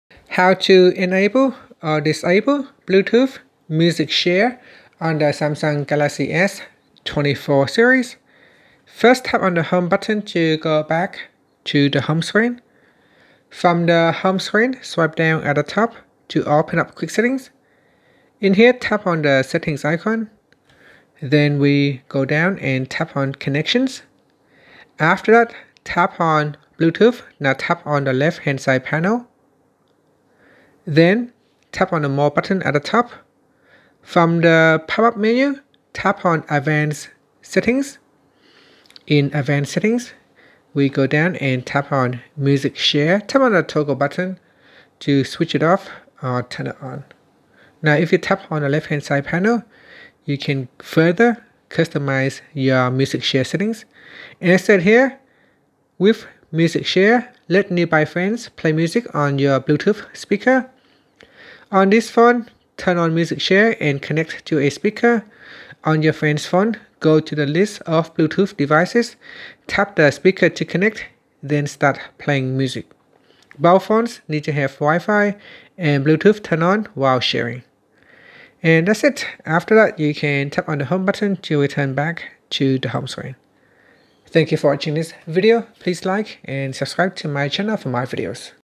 Galaxy S24/S24+/Ultra: How to Enable/Disable Bluetooth Music Share
In today's tutorial, we'll walk you through the steps to enable or disable Bluetooth Music Share on your Samsung Galaxy S24, S24+, or Ultra. Whether you want to share your favorite tunes with friends or keep your music to yourself, Bluetooth Music Share offers convenience and control. Join us as we provide easy-to-follow instructions for managing this feature on your Galaxy device.